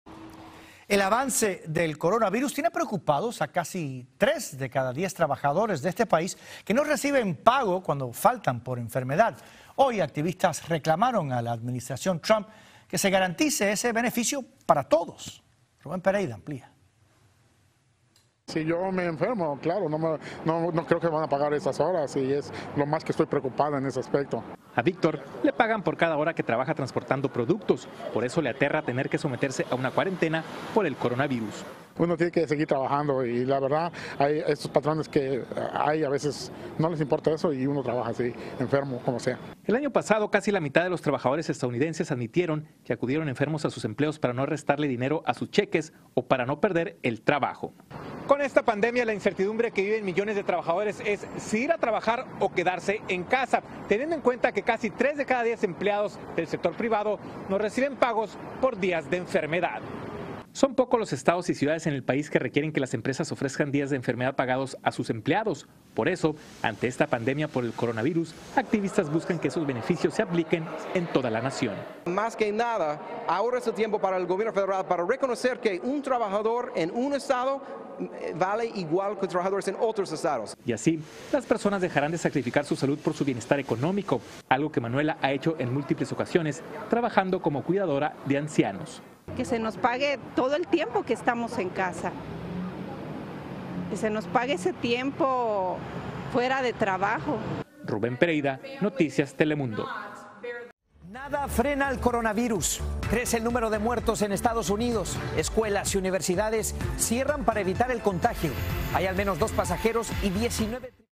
Preocupa la posible falta de ingresos a empleados que contraigan COVID-19 | Noticias Telemundo
Video oficial de Noticias Telemundo. Organizaciones que velan por los derechos de los trabajadores están sonando las alarmas e hicieron un llamado al gobierno federal para evitar que los empleados sean despedidos o pierdan ingresos por no poder asistir al trabajo durante una cuarentena.